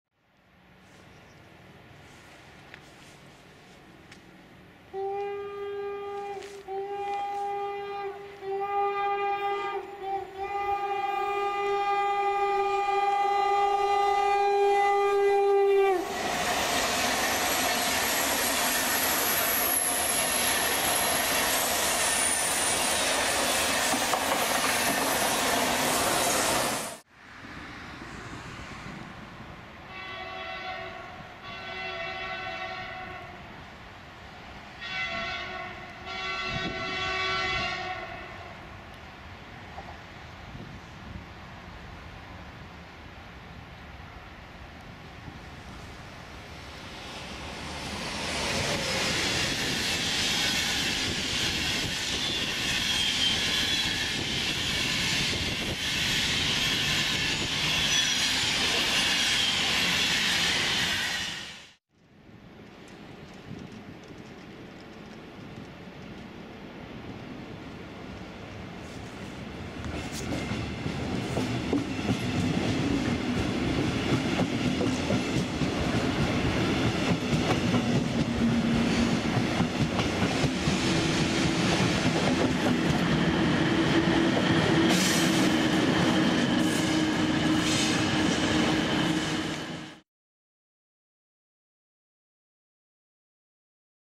Der wohl lauteste Tf Hamburgs! | MegaMakros IC Steuerwagen und BR101 | 27.12.2015
Danke an die netten Tf's!!!

Bis denn...

EFN - Dominik